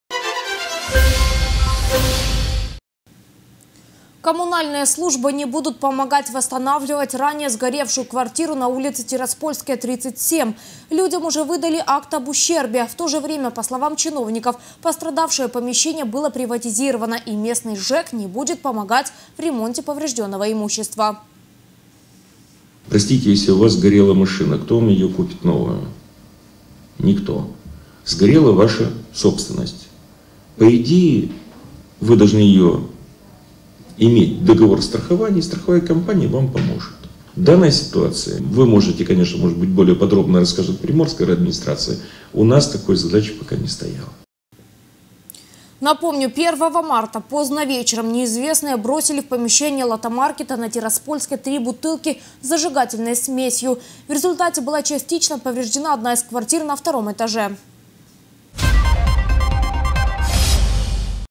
Коммунальные службы не будут помогать восстанавливать сгоревшую квартиру на Тираспольской
Людям уже выдали акт об ущербе. В тоже время, по словам чиновников, пострадавшее помещение было  приватизировано и местный ЖЭК не будет помогать в ремонте поврежденного имущества...